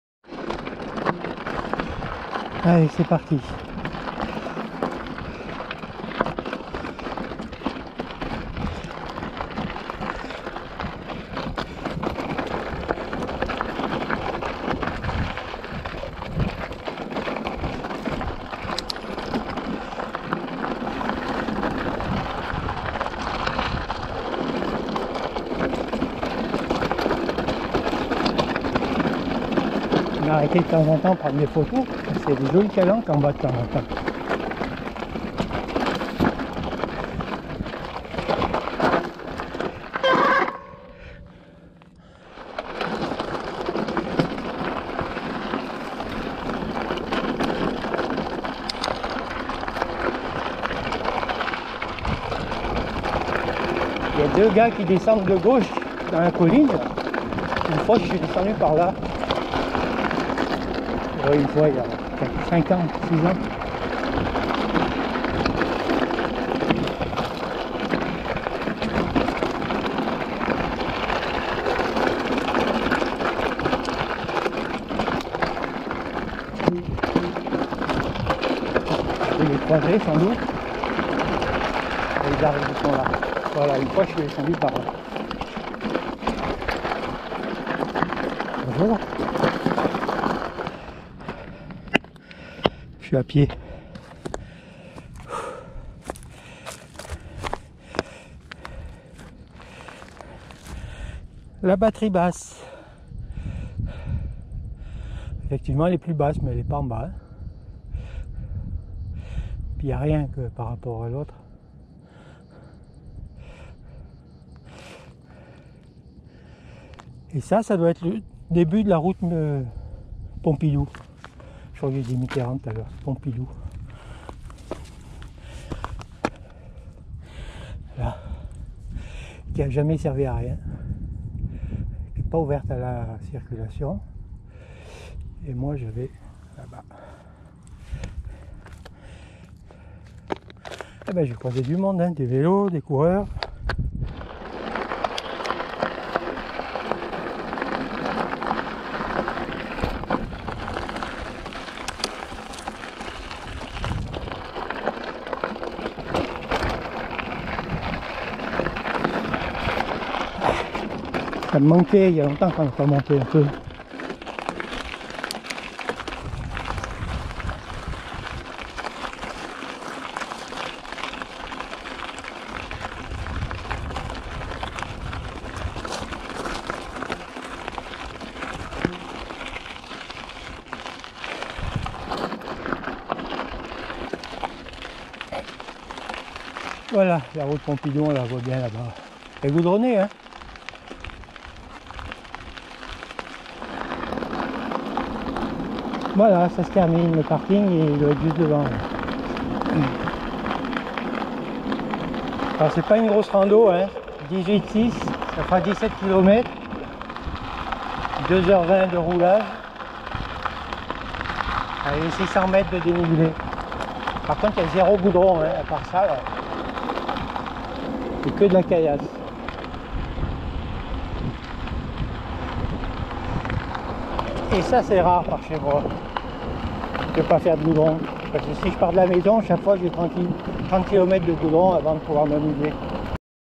erevine niolon la vesse 08 2020 part 8
à partir du rove,le vallon de l'everine,la batterie haute de niolon,la vesse et le fort de figuerolles et remontée par la route pompidou